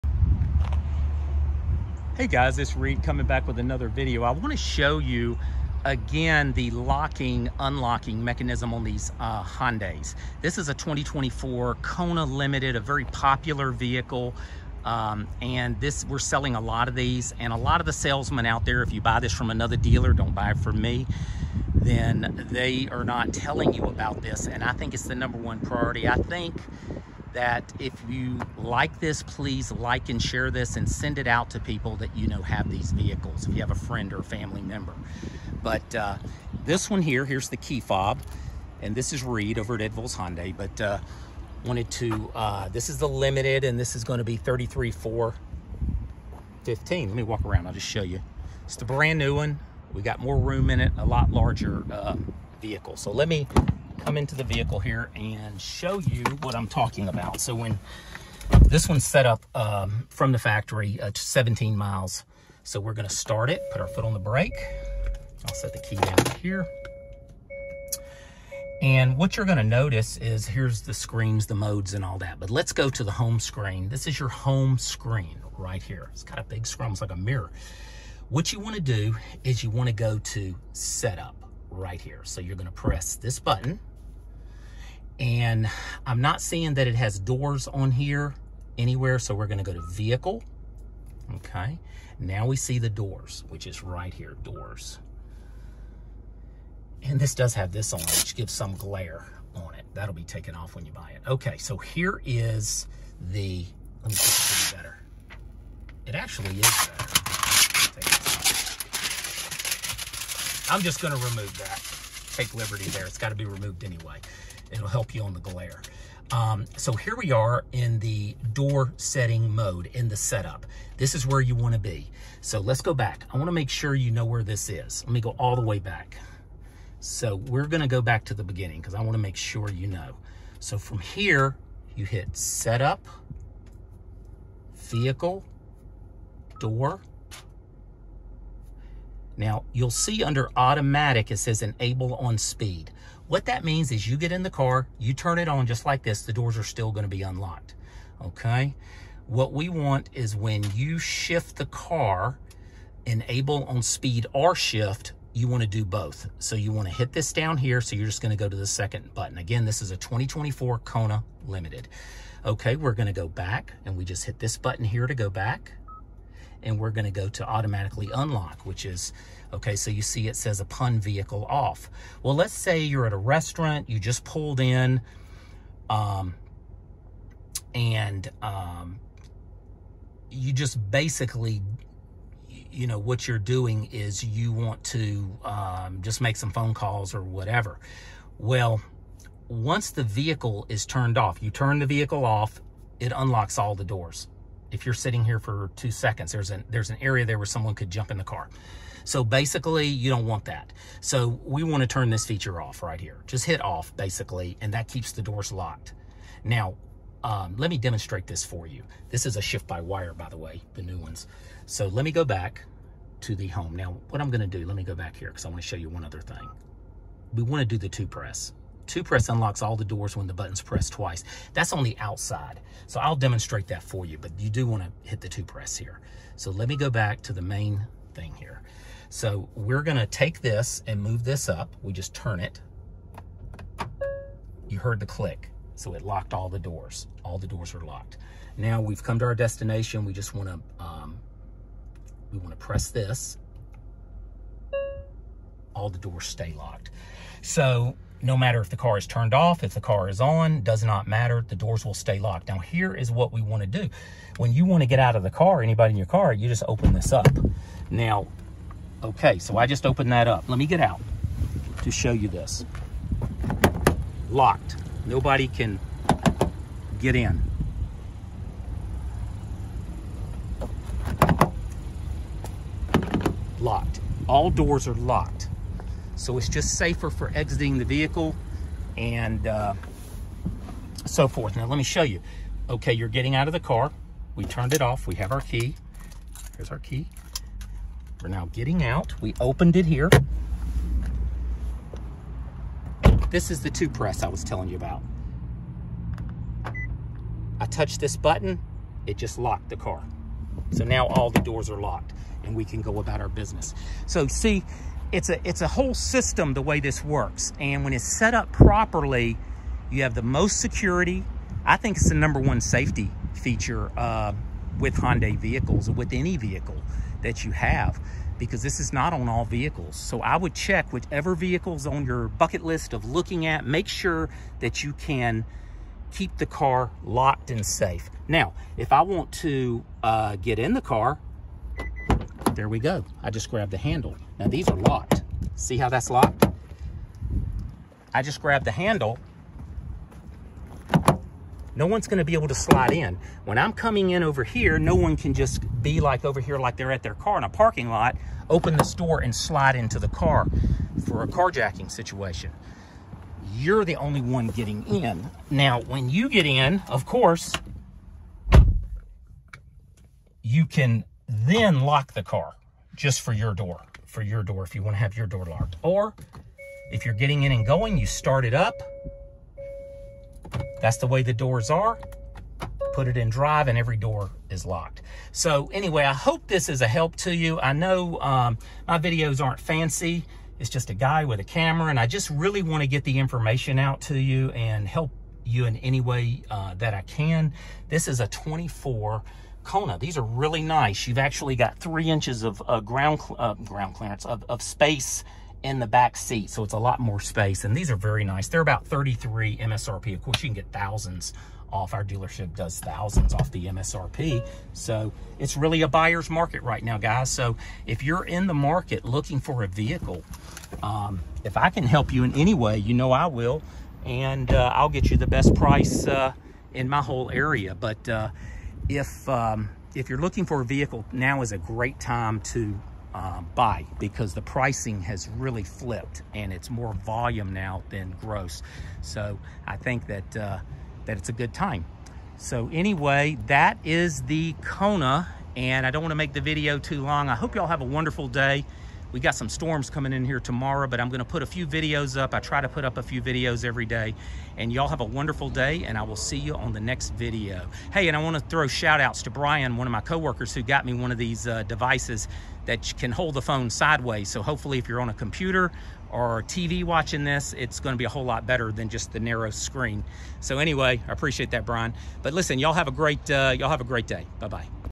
2024 Kona Limited #1 Safety Feature to turn on Now! Will be the same on other 2024 Kona
This is a HOW TO video on how to set the door lock and unlock safety on the new 2024 Kona Limited but will be the same or similar on other trim levels. I believe this is the #1 safety feature you can change in the settings yourself and i will show you how easily and quickly 😊